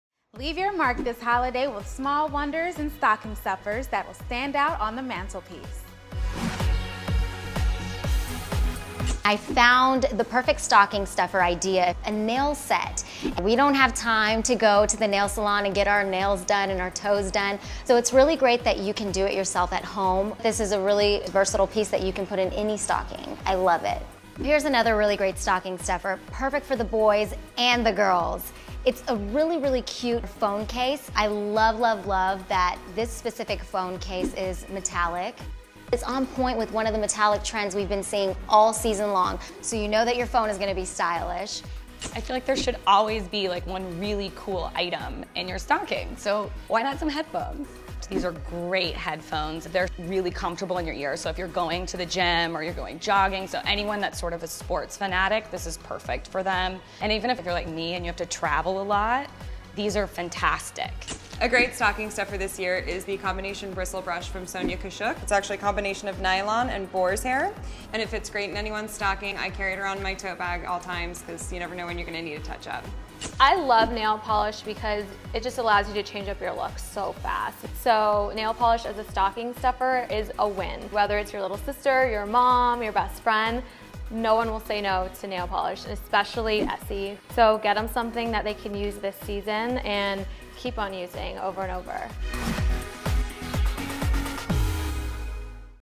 Stocking Stuffers for Everyone on Your List | Style Squad
These festive finds are small enough to fit in holiday stockings yet big enough to leave a lasting impression. Find the perfect stocking stuffers at Target with our Glam bloggers Sazan Barzani, Lynsey Eaton, Rachael Dickhute, and Ashley Torres.